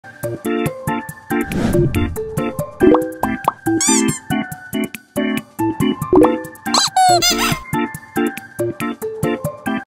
What're those? 2 cutest sleepy Westies
Two of my West Highland Terrier are sleeping